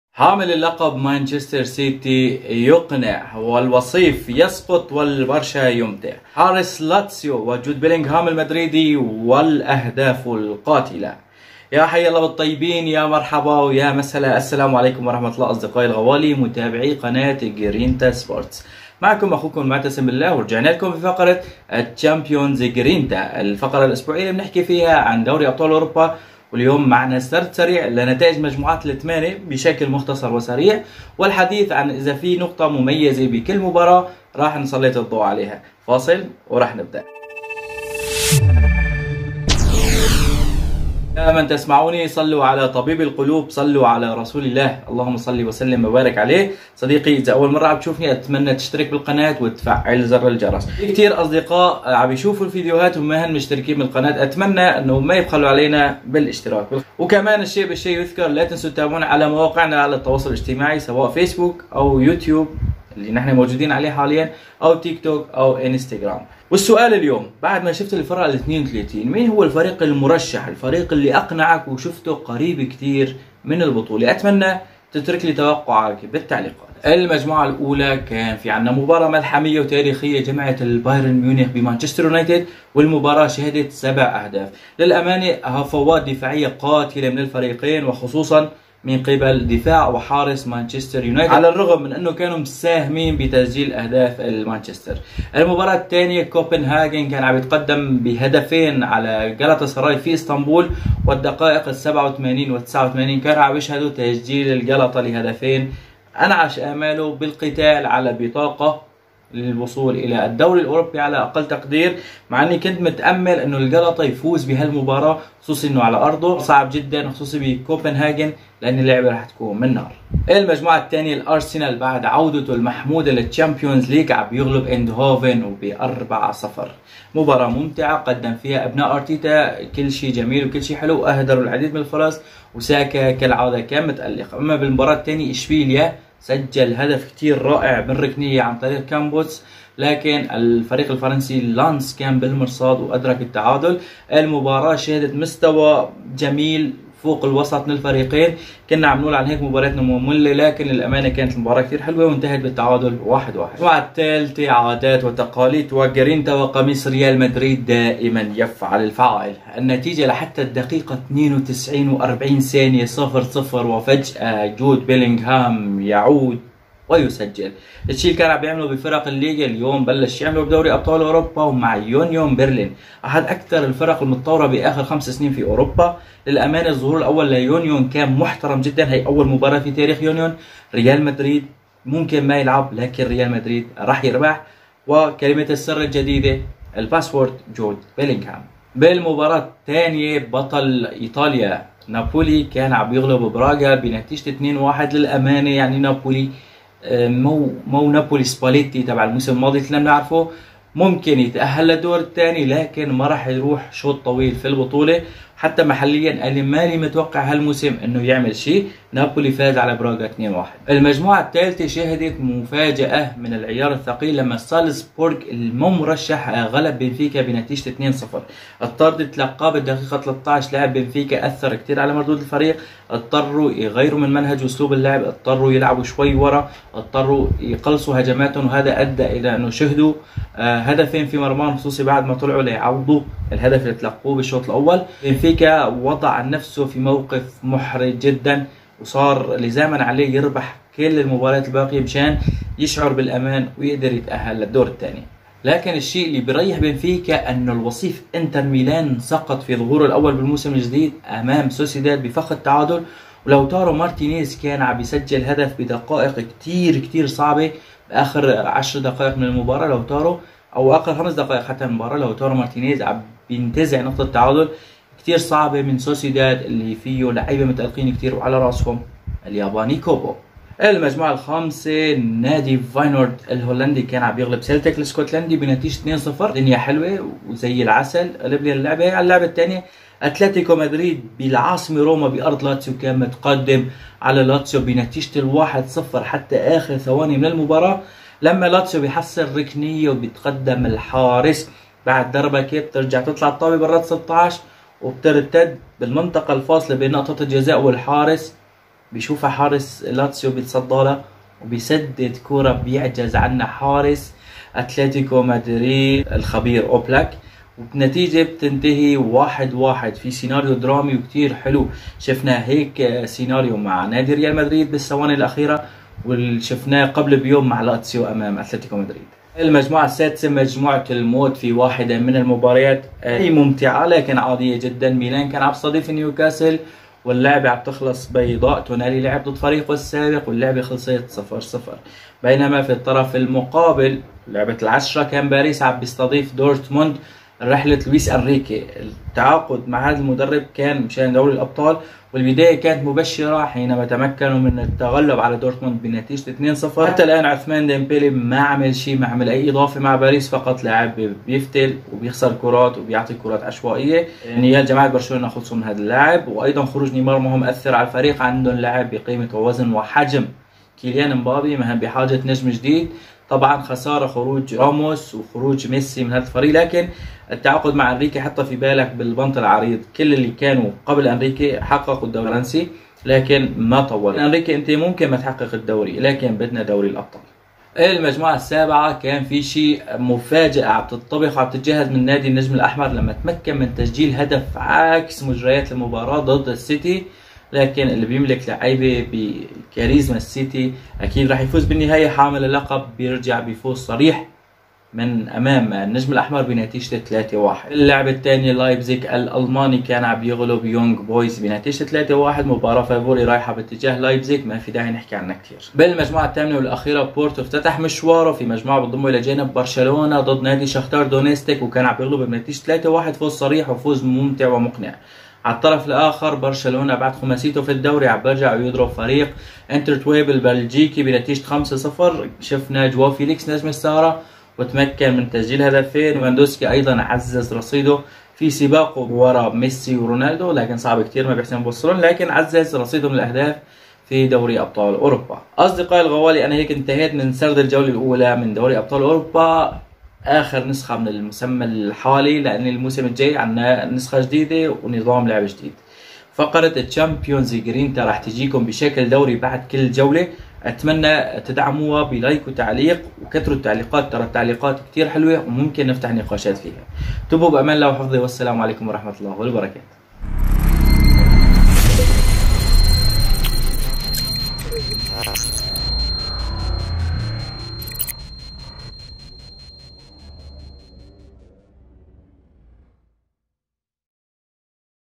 التشامبيونز غرينتا - البرشا و الغانرز الأمتع - الريال و لاتسيو اللحظات الأخيرة
📍البرشا بخماسية ع انتريوب البلجيكي
📍الريال و لاتسيو اللحظات الأخيرة
📍 ساكا و جود و فيليكس نجوم الجولة الأولى